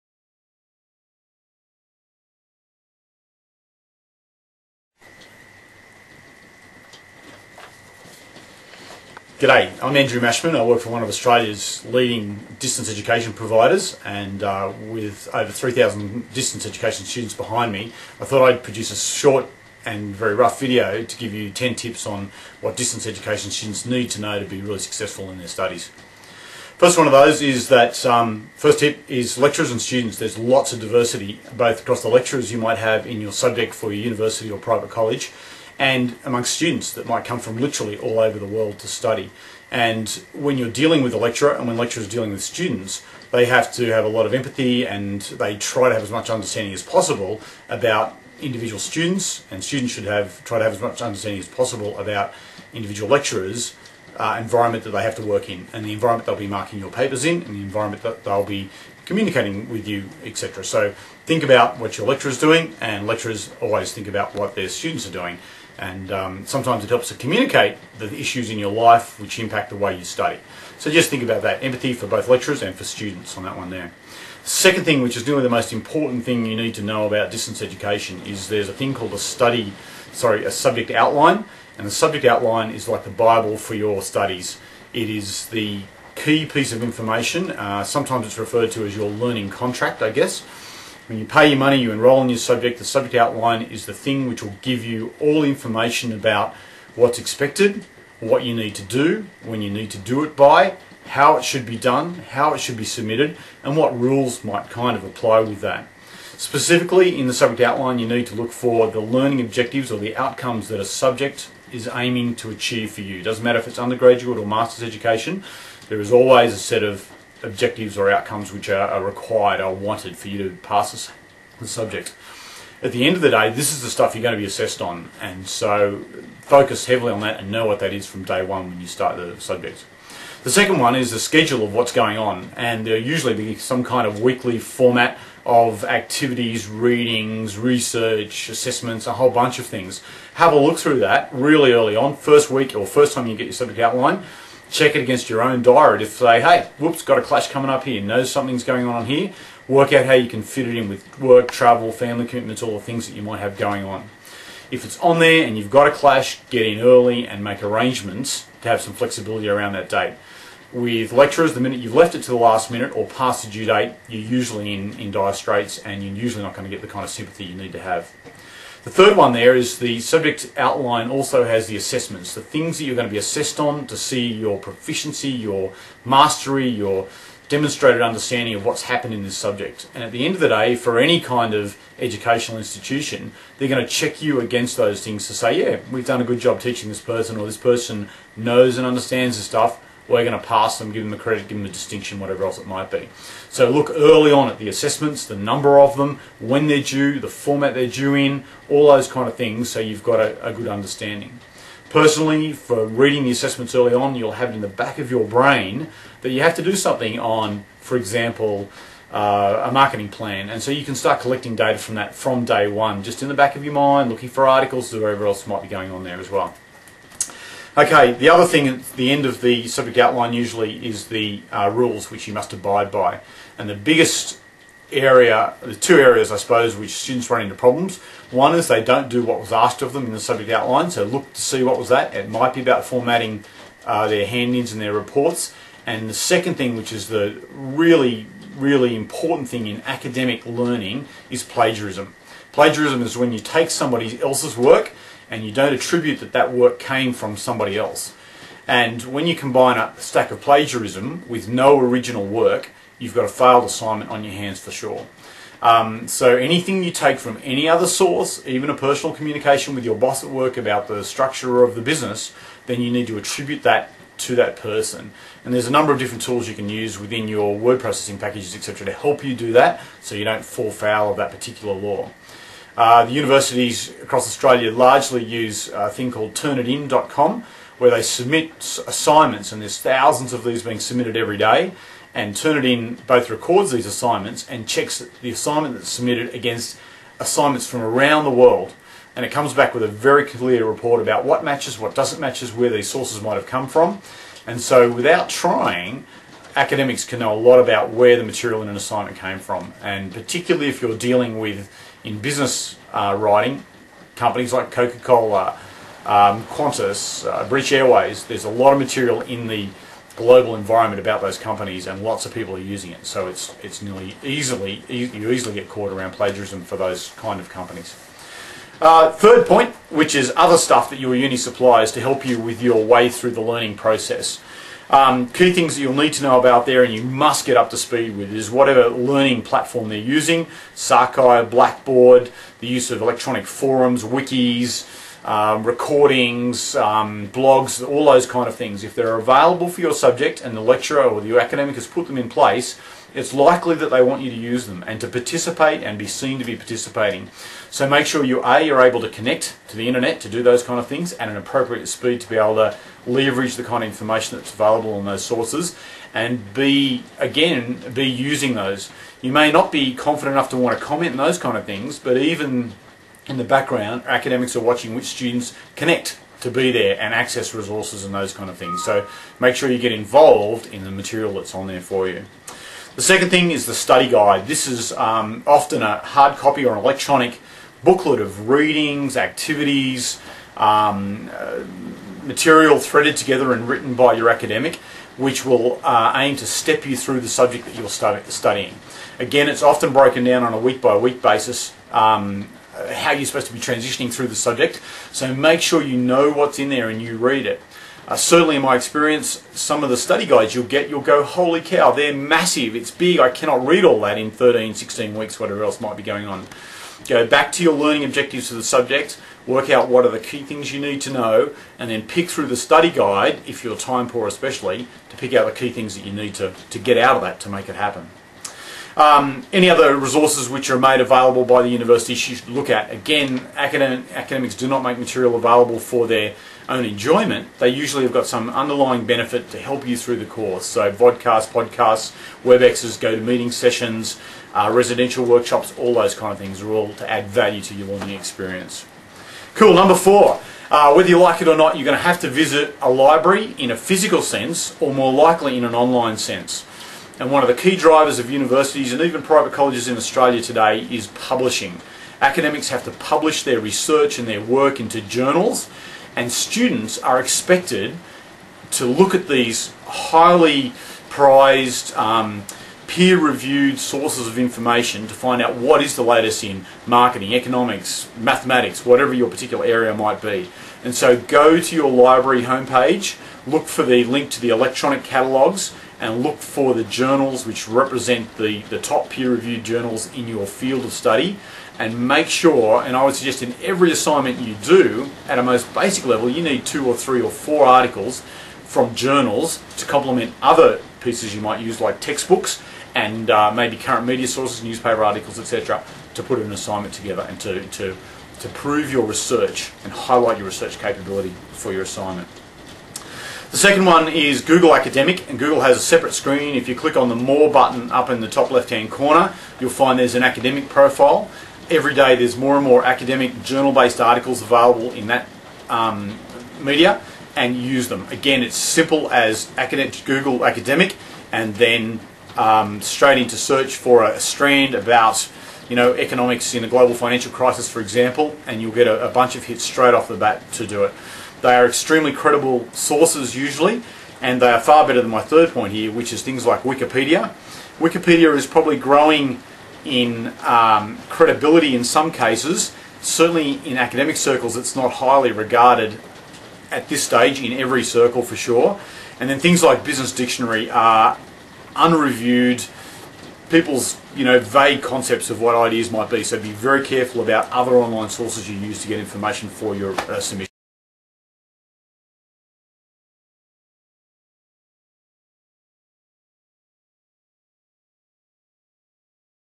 10 things to know about distance education as a student part 1
10 Things Students should know if they are starting Distance Education in Australia that will help focus their efforts and improve their success. This is a 2 part video